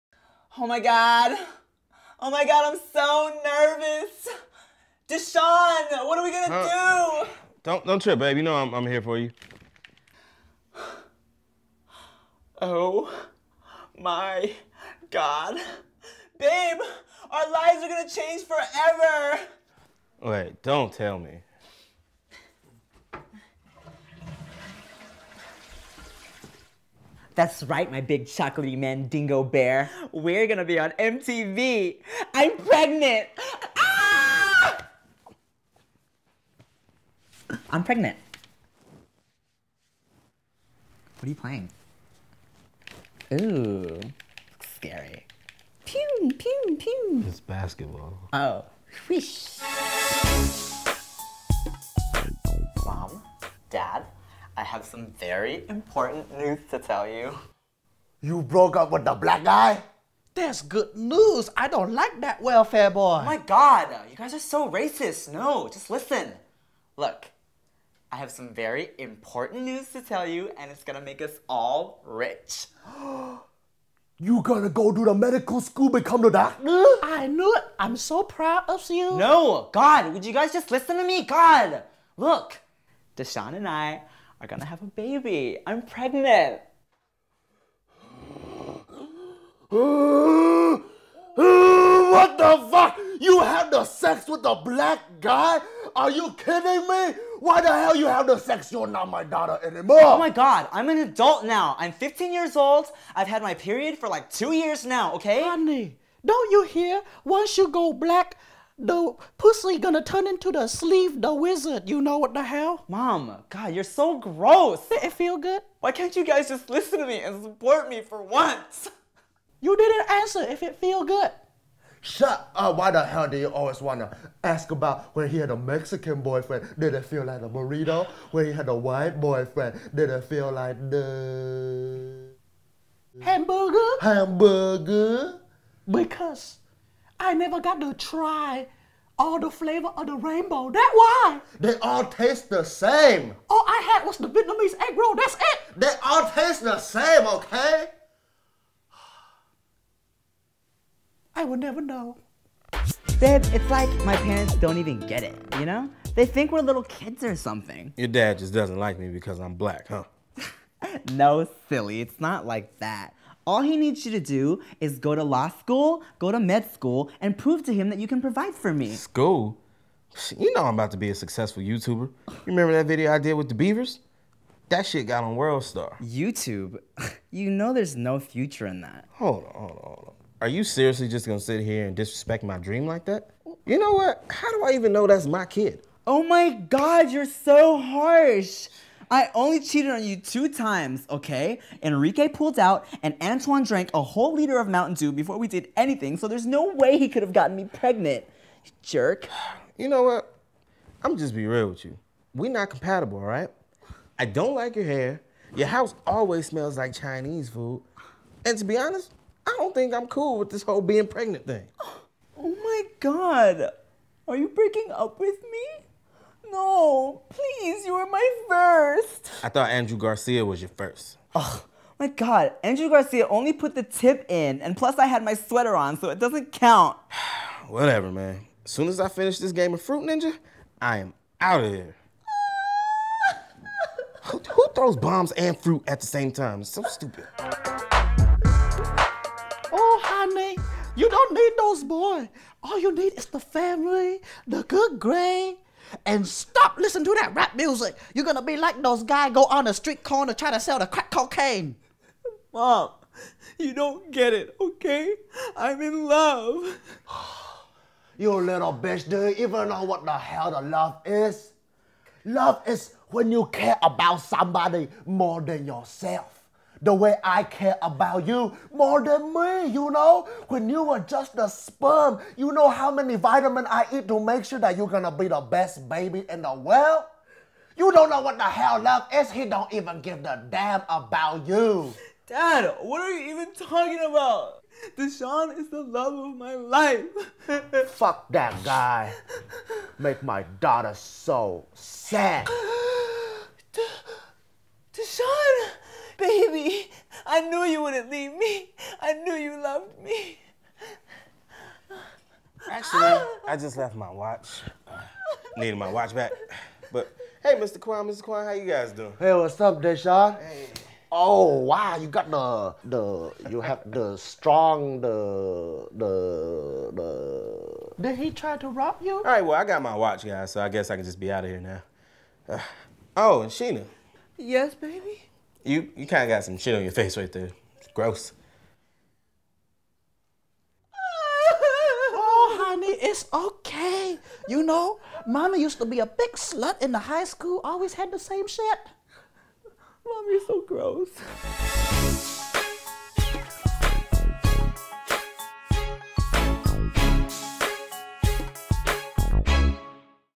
15 & PREGNANT
Tim does a parody on 16 & Pregnant.

Crew:

Written by: Timothy DeLaGhetto

Executive Producer: Mickey Meyer

Production Coordinator: George Kimmel IV

DP: Mike Bauman

AD/Gaffer: Trent Turner

Sound: Sheena Moriartiy